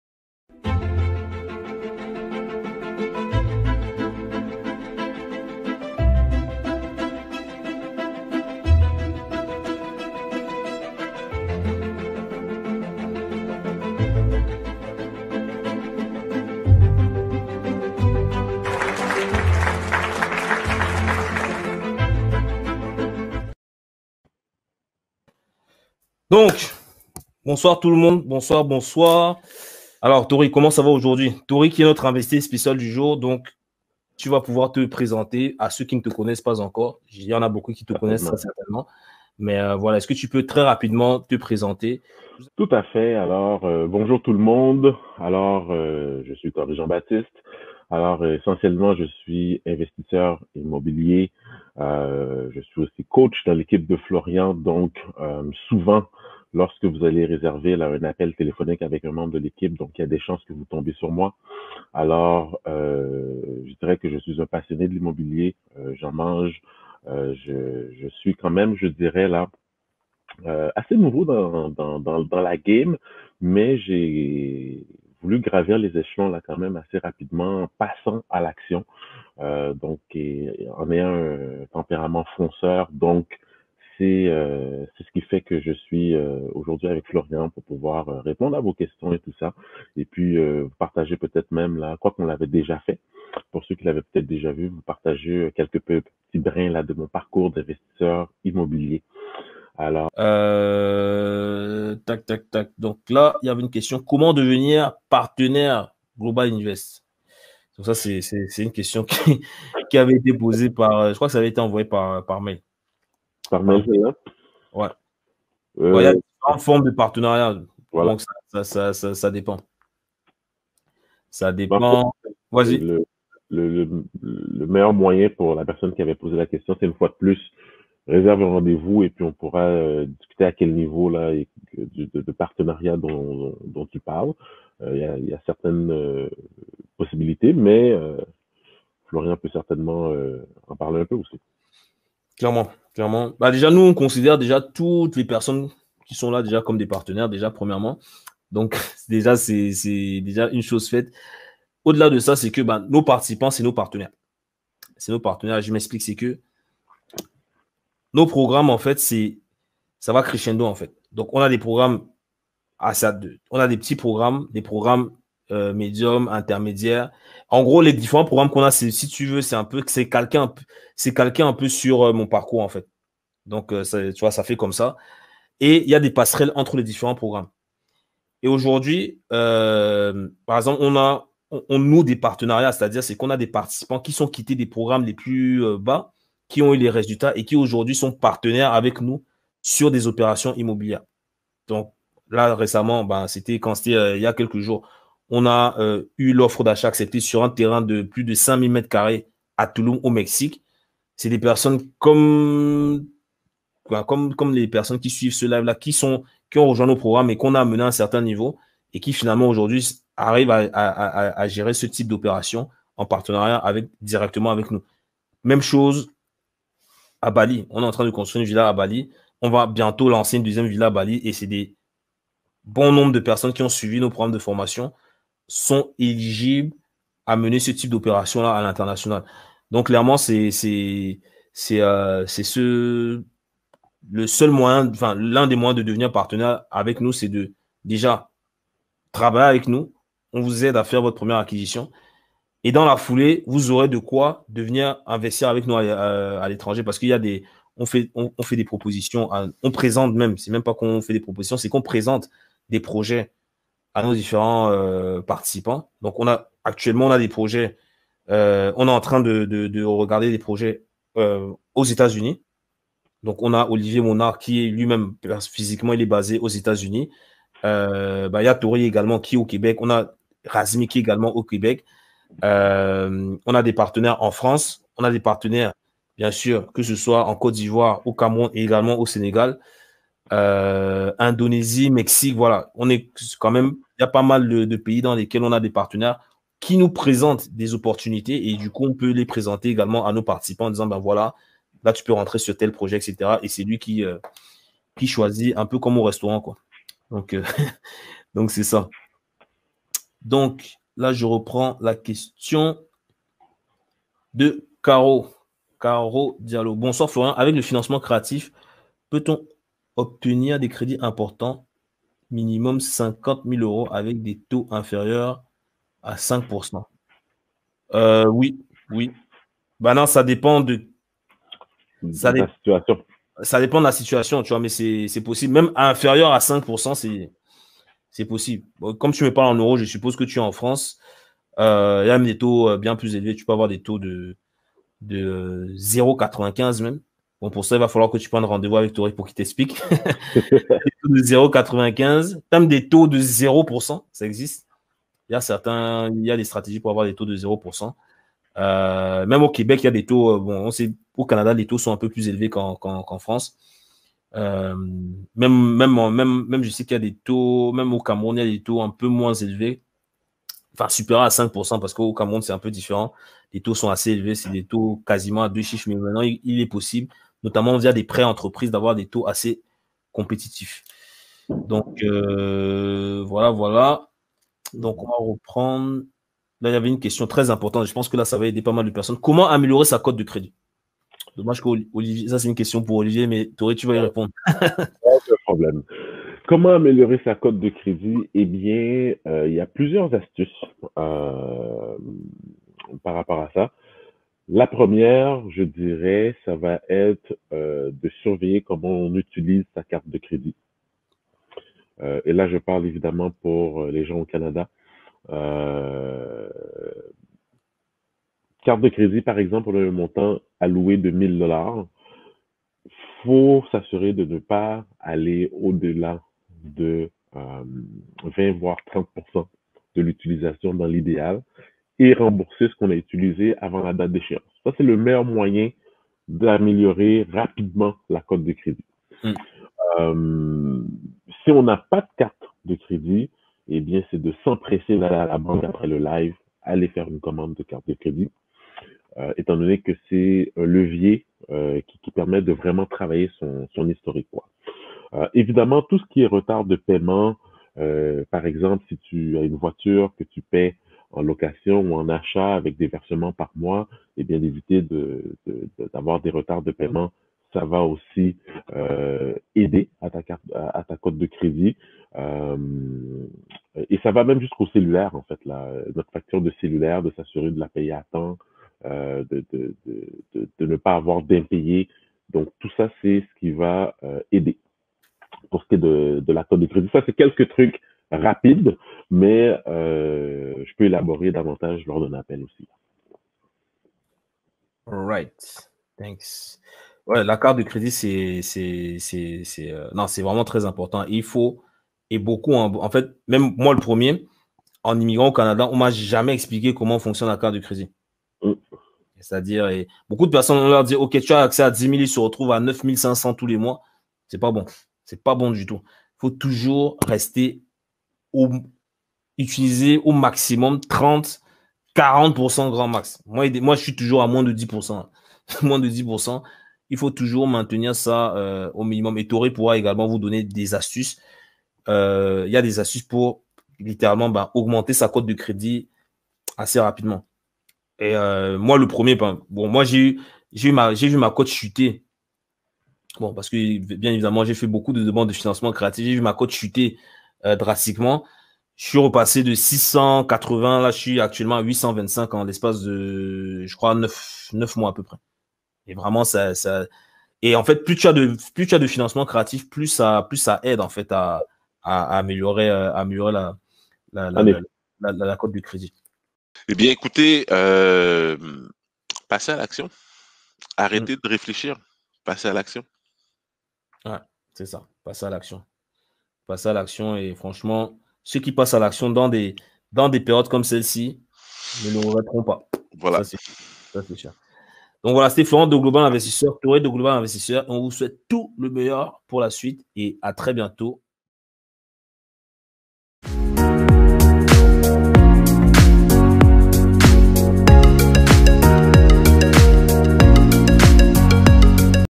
Le Seul Moyen De Réussir En Immobilier - Live Avec Torry Jean Baptise
Dans cette nouvelle vidéo live avec Torry Jean Baptiste on te partage le seul moyen de réussir en immobilier.

Avertissement